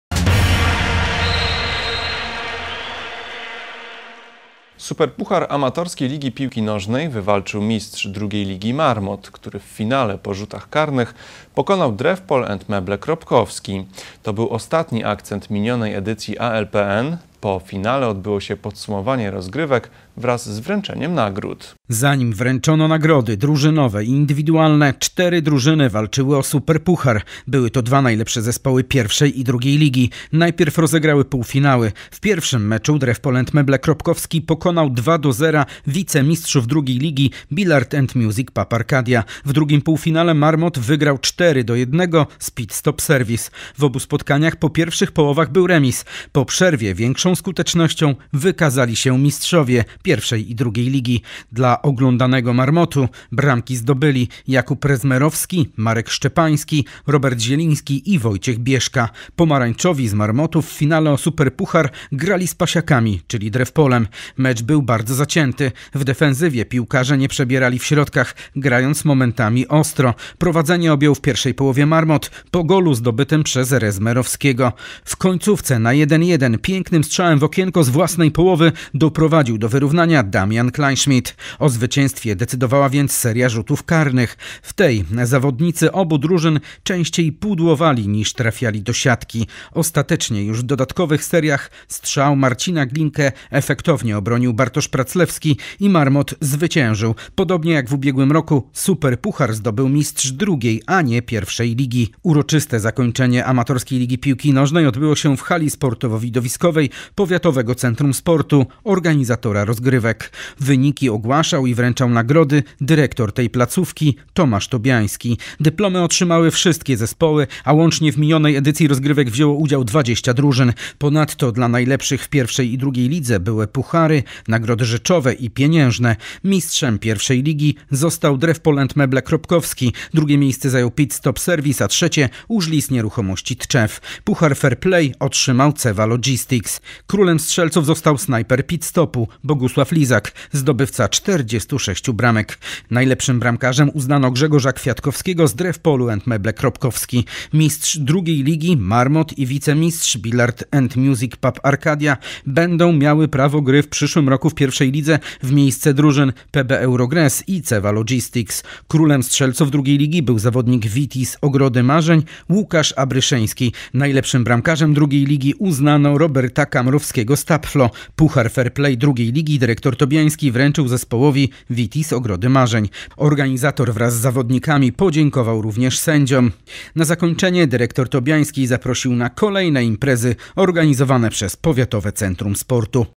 Superpuchar ALPN - Tv Tetka Tczew HD
Superpuchar Amatorskiej Ligi Piłki Nożnej wywalczył mistrz drugiej ligi Mar Mot, który w finale po rzutach karnych pokonał Drewpol & Meble Kropkowski. To był ostatni akcent minionej edycji ALPN. Po finale odbyło się podsumowanie rozgrywek wraz z wręczeniem nagród.